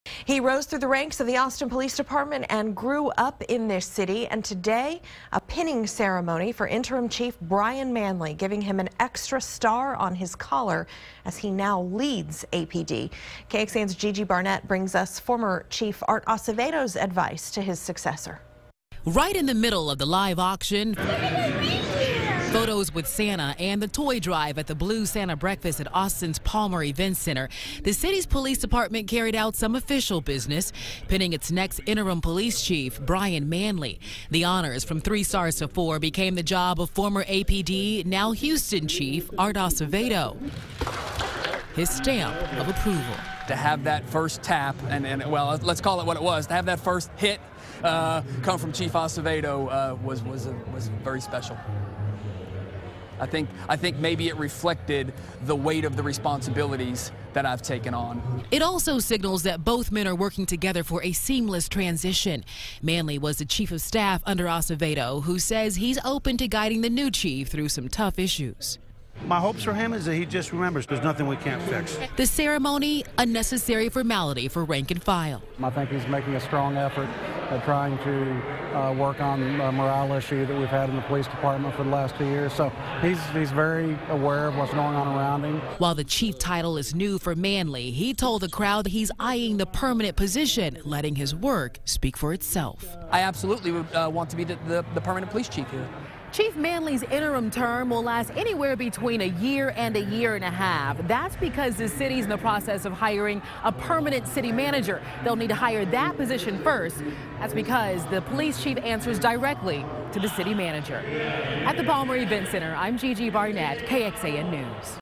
Operation Blue Santa asks community to give back for the holidays
The Austin Police Department will once again hand out toys and food to families in need as a part of Operation Blue Santa.